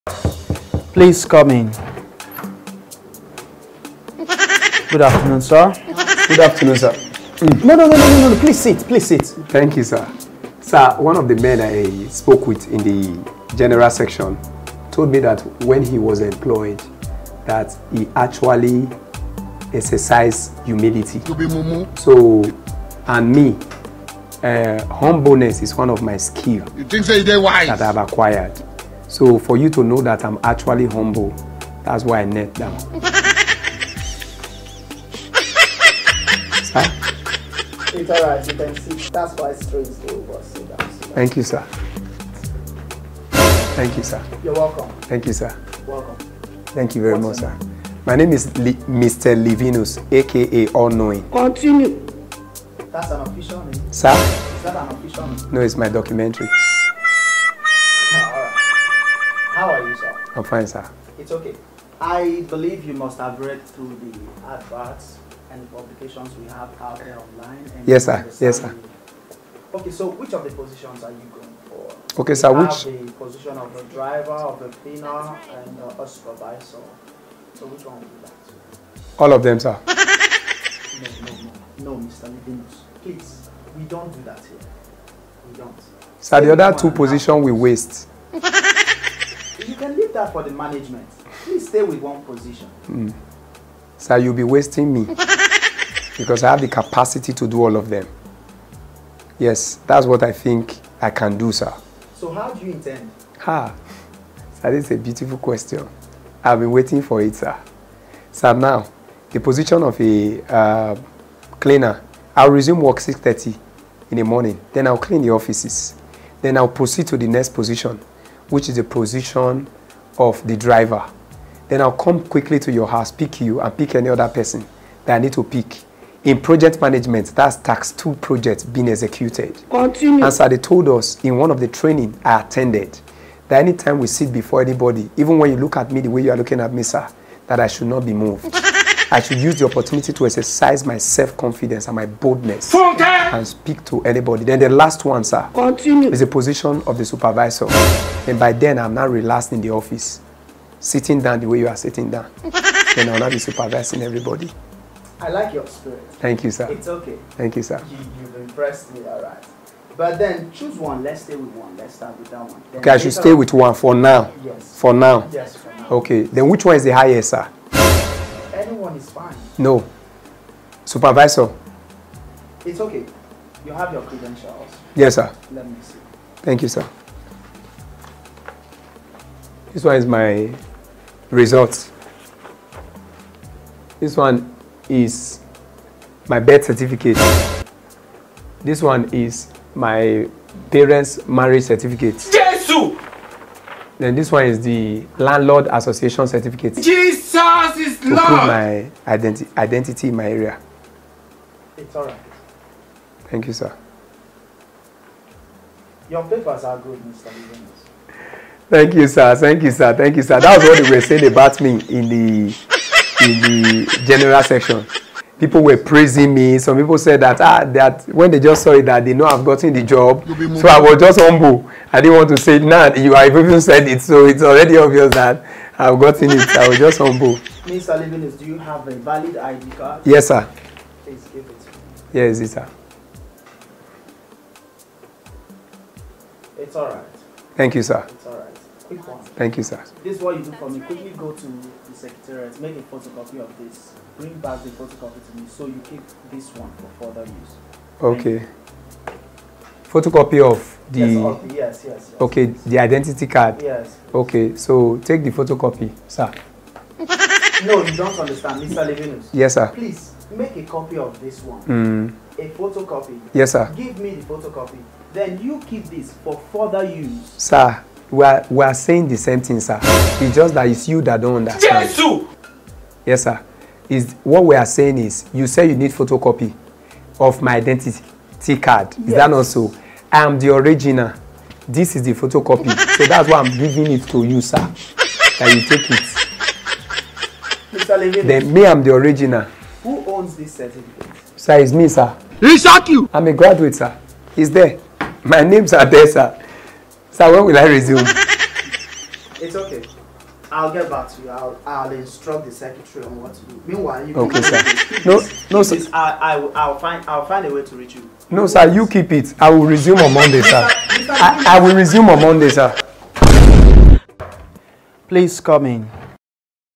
I AM THE ORIGINAL (ORIGINAL VS PHOTOCOPY ID JOB INtERVIEW )
Job interview, the life of Mr.Livinus. Original Identity card Vs Photocopy photocopy. There was a missed up base on mental short sightedness from the part of Mr. Livinus Watch and say what you think.